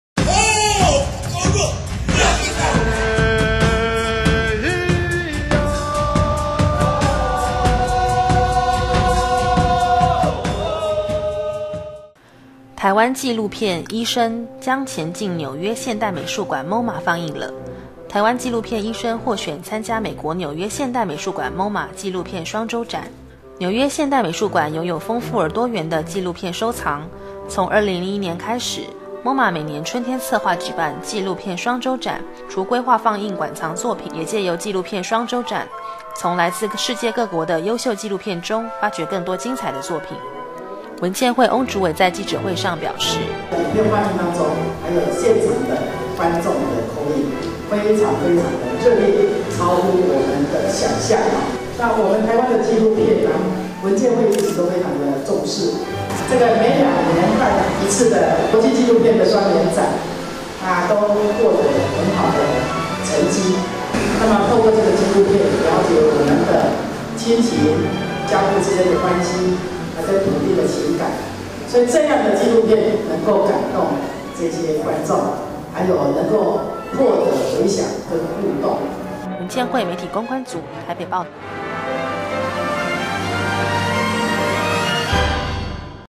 文建會-「前進紐約MoMa」台灣紀錄片參展
台灣紀錄片「醫生」獲選參加美國紐約現代美術館(MoMA)「紀錄片雙週展」，行政元文建會在昨(30)日召開記者會與國人分享這項喜訊。行政院文建會主任委員翁金珠、文建會第三處處長黃才郎、文建會所屬國立台灣美術館館長薛保瑕、《醫生》副導演藍雅玲、《黑暗世界》導演周美玲、《景福門日記》導演李家驊皆出席記者會。

文建會翁主委在記者會上表示，文建會長期以來關注台灣紀錄片發展，為鼓勵台灣紀錄片從業人員並培育觀眾，去年委由國美館規劃「HOT!紀錄片，大家緊來看!」台灣國際紀錄片巡迴放映活動，獲得許多台灣民眾的支持與參與。這次經由本會駐紐約台北文化中心及國美館的努力，讓台灣精采的紀錄片有機會參與重要的紀錄片影展。希望能透過這些優秀台灣紀錄片，將台灣獨特的文化視野與世界分享，並協助更多優秀的台灣紀錄片工作者登上國際舞台。

紐約現代美術館擁有豐富而完整的紀錄片收藏，對於紀錄片發展的趨勢亦有獨到的見解。從2001年開始，MoMA每年春天策劃舉辦「紀錄片雙週展」，除規劃放映館藏作品，也藉由「紀錄片雙週展」從來自世界各國的優秀紀錄片中，發掘更多精采的作品。

本次獲選參與MoMA雙週展的台灣紀錄片「醫生」是導演鍾孟宏的作品，也是2006年台灣國際紀錄片雙年展「公視獎」觀眾票選最佳影片，其內容主要是敘述一位來自台灣竹東的溫醫師與妻兒長居美國，在美國行醫期間，偶然因為一位前來求診的十二歲秘魯男孩，而開啟這段「醫生」的紀錄片奇緣。

推薦「醫生」一片參展的國立台灣美術館表示，2006年國立台灣美術館舉行紀錄片雙年展中曾安排放映紐約現代美術館館藏紀錄片特展，並邀「紀錄片雙週展」策展人莎莉柏格(Sally Berger)來台參加雙年展，藉機觀摩台灣具代表性的紀錄片作品，也促成此次「醫生」一片參展的契機。

此外，在紐約雙週影展後，為持續推廣台灣紀錄片，本會紐約台北文化中心將辦理「看見台灣」紀錄片大賞，除了放映「醫生」一片外，另外介紹2006台灣國際紀錄片雙年展的開幕片「大台中紀事」、2006台灣國際紀錄片雙年展「台灣獎」首獎的「黃屋手記」、2006台灣國際紀錄片雙年展「台灣獎」優等獎的「黑暗世界」與2006台灣國際紀錄片雙年展「台灣獎」評審特別推薦的「景福門日記」。